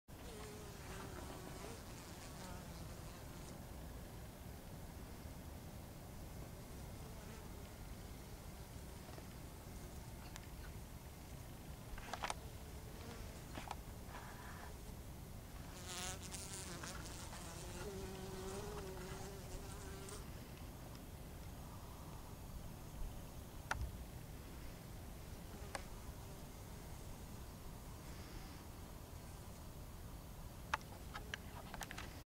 Geapan, bottinatrici "rinarniano" con polline corbezzolo
Geapan, bottinatrici "rinarniano" con polline corbezzolo, a Framura, fine autunno 2012